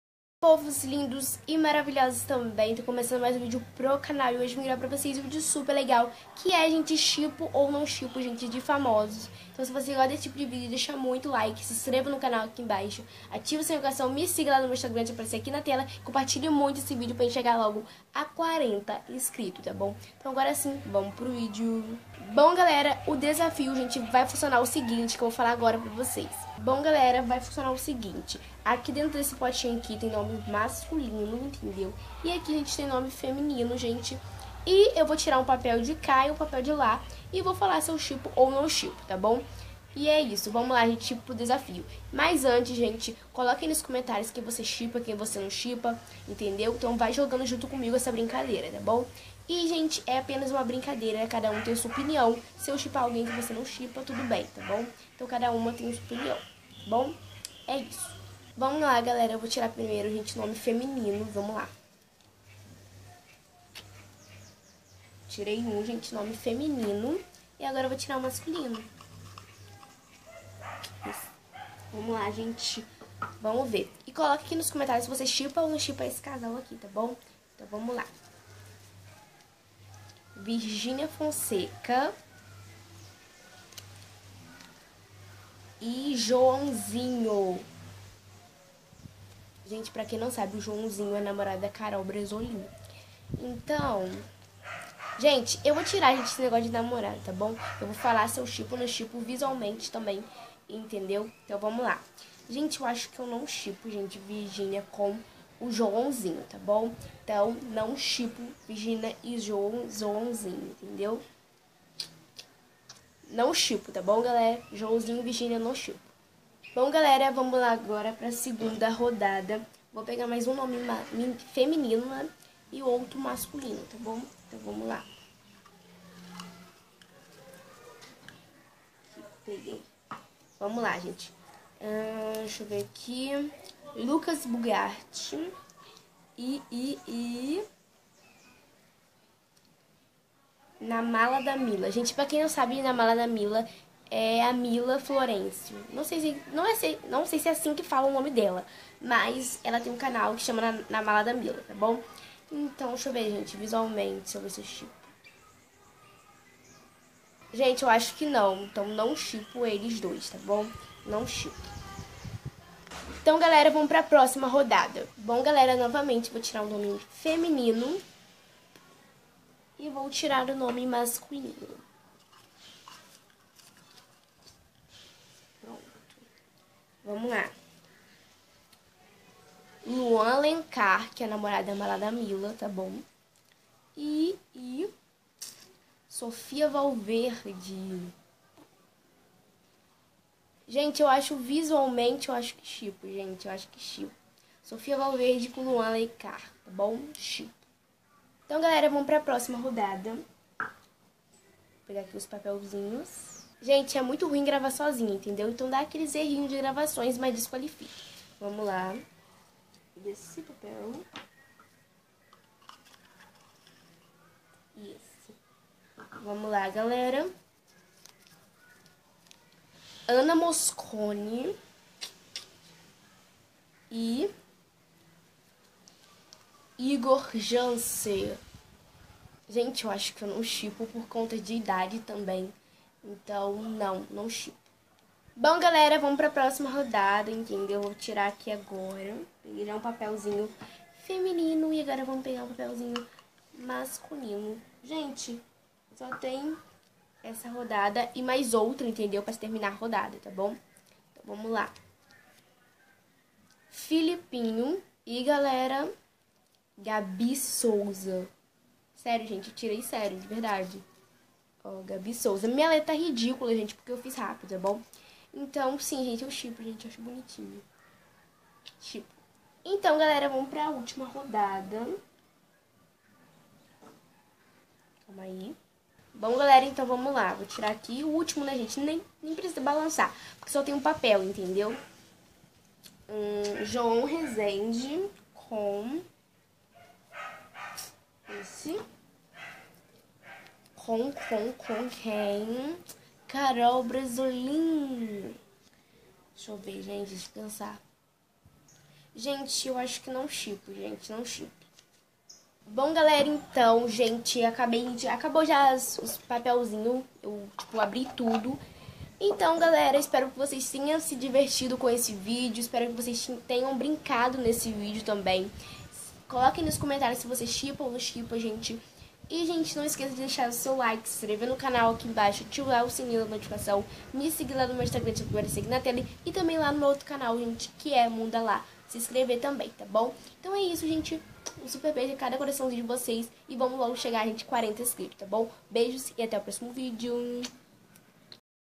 Shippo ou não shippo com nomes de famosos!!
Fala meu povos lindos e maravilhosos também, hoje vim gravar para vocês shippo ou não shippo com nomes de famosos.

Outro canal:mundo da lá

Bjss até o próximo vídeo!!